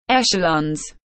How to pronounce Echelons / Echelons pronunciation
Raccoon teaches you how to pronounce Echelons . Raccoon vous apprends a prononcer Echelons . Como pronunciar Echelons